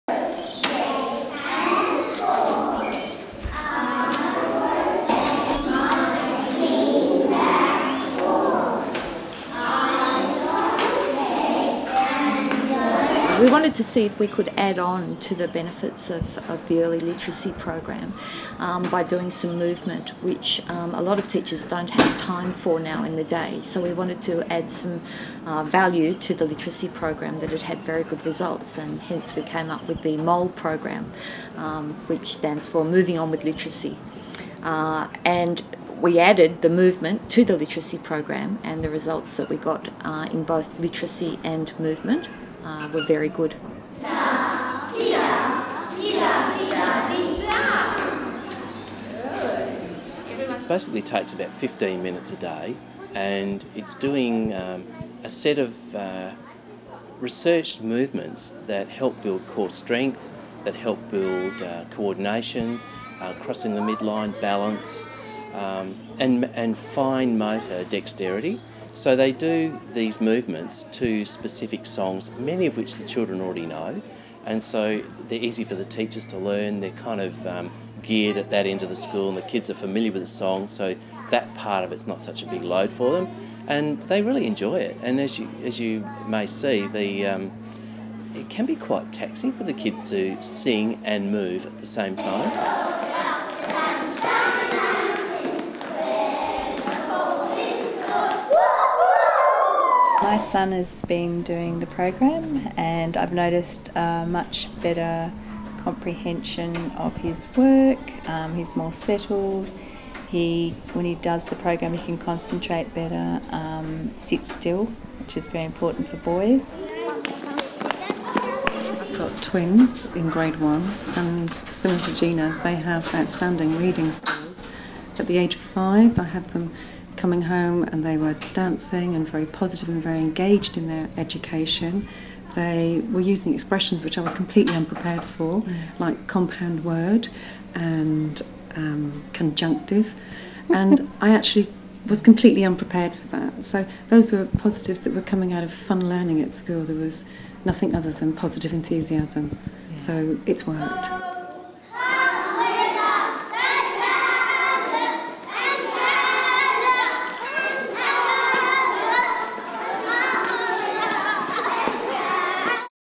Applecross Primary School students improve their literacy through movement
Applecross Primary School students are using the ground-breaking Moving On With Literacy program to improve their reading and writing skills